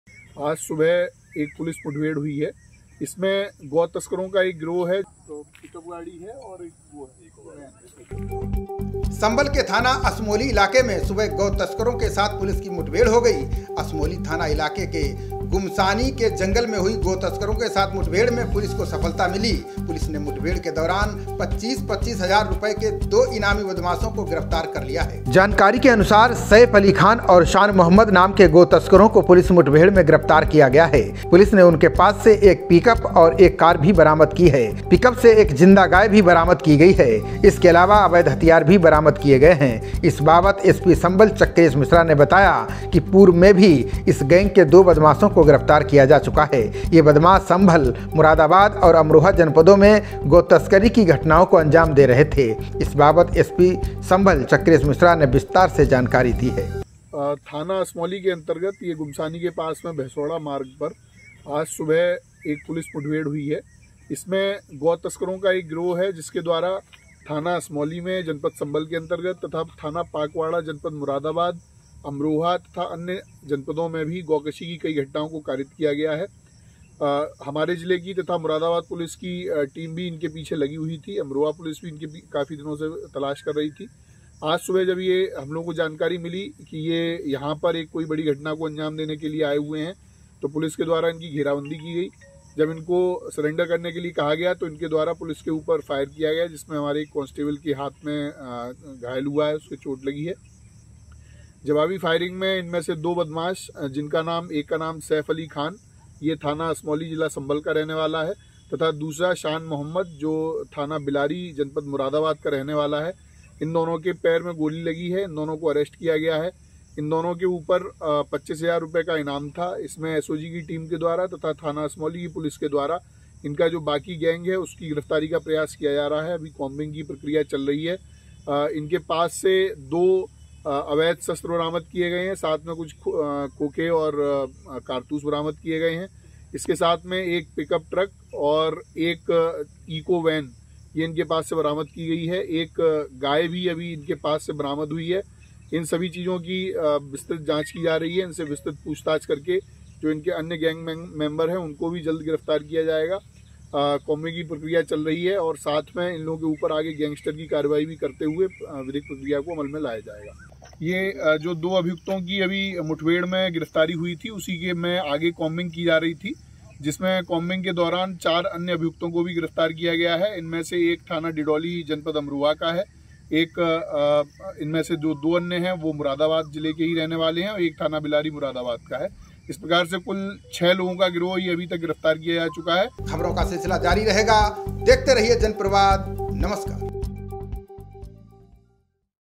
GAU TASKAR : पुलिस मुठभेड़ में गो-तस्कर गिरफ्तार | 25-25 हजार का रखा गया था इनाम | Sambhal |Janpravad
संभल पुलिस को मिली सफलता
दो गोतस्करों को किया गिरफ्तार
25-25 हजार के इनामी थे अपराधी
गैंग पर पहले भी हो चुकी है कार्रवाई
मुठभेड़ में पुलिस का जवान भी घायल
जीवित गाय को भी किया गया बरामद
एसपी संभल चक्रेश मिश्रा ने दी जानकारी

Janpravad Magazine
Janpravad Tourism
Janpravad Superfast